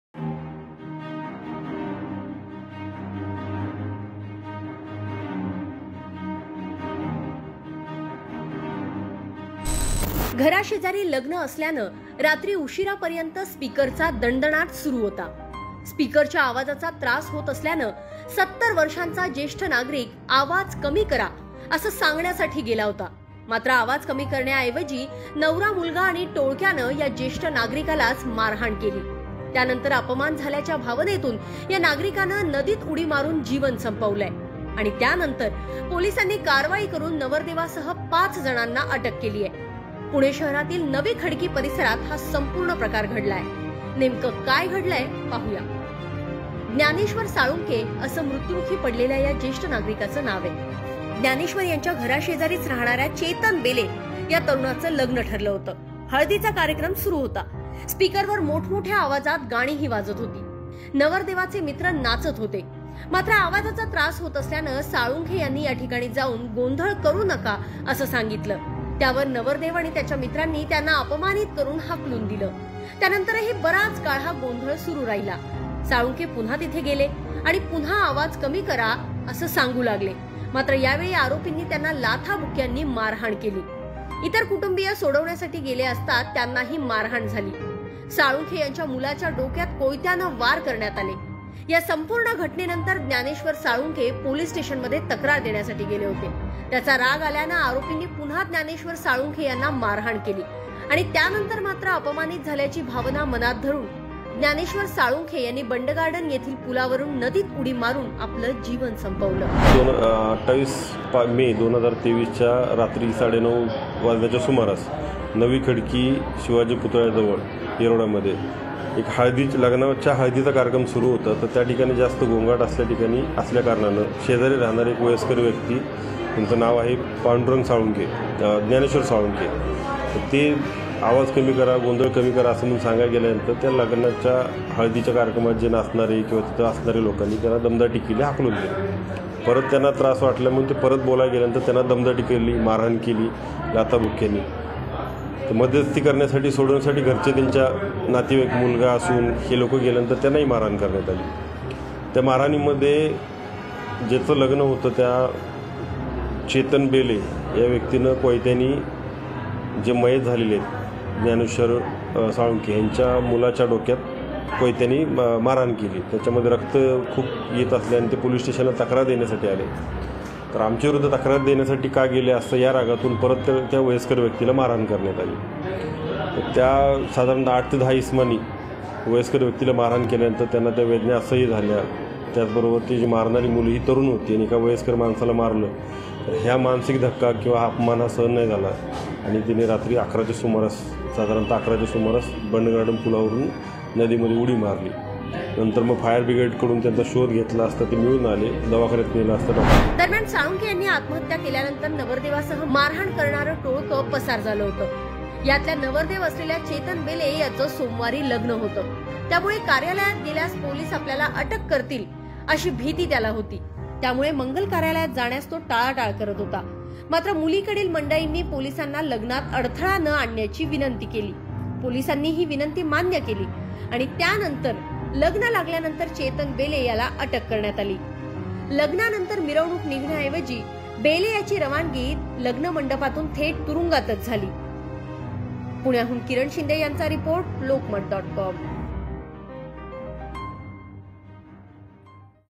Pune : अपमानित झाल्याने वृद्धाचे टोकाचे पाऊल; लग्न मंडपातून नवरदेवाची रवानगी थेट तुरुंगात | KI3
घराशेजारी लग्न असल्याने रात्री उशिरापर्यंत स्पीकरचा दणदणाट सुरू.. स्पीकरच्या आवाजाचा त्रास होत असल्याने 70 वर्षाचा ज्येष्ठ नागरिक आवाज कमी करा सांगण्यासाठी गेला होता.. मात्र आवाज कमी करणे ऐवजी नवरा मुलगा आणि टोळक्यानं या ज्येष्ठ नागरिकालाच ... केली..